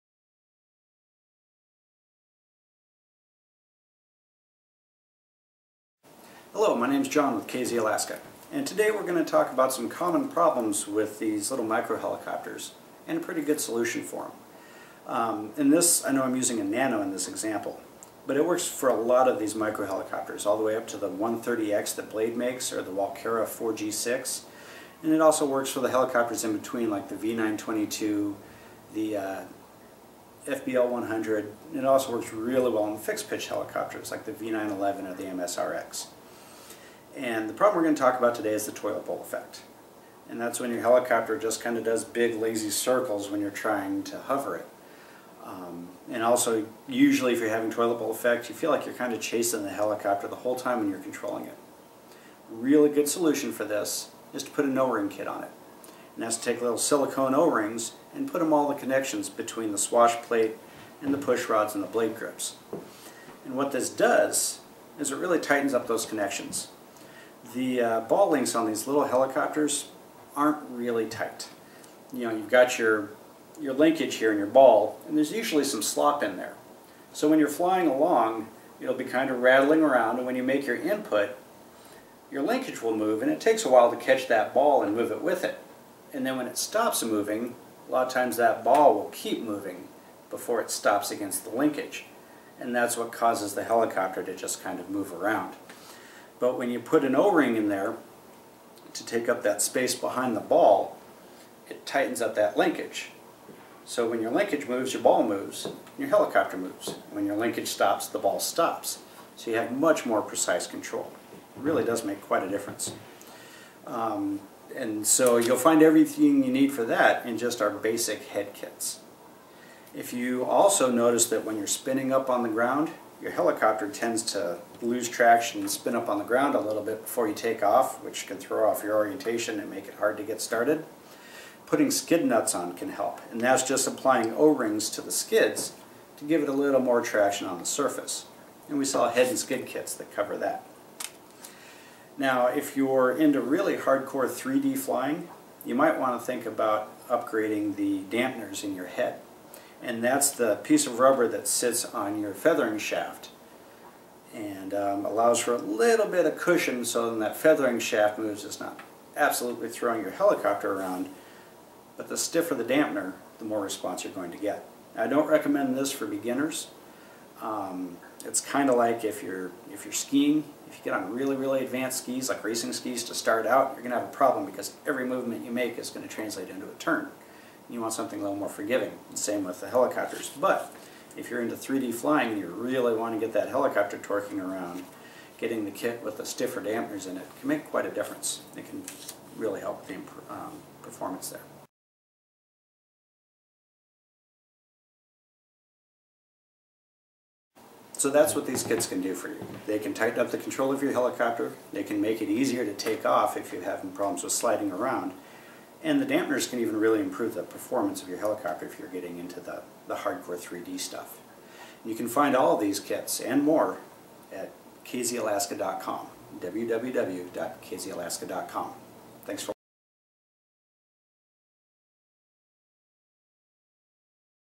Micro-Helicopter O-ring Upgrades
Eliminate toilet bowl effect with o-rings.  Check out our Solid Carbon Fiber Tail Booms (Square booms and Round booms) and our KZ Tail with Plug N' Play Motor!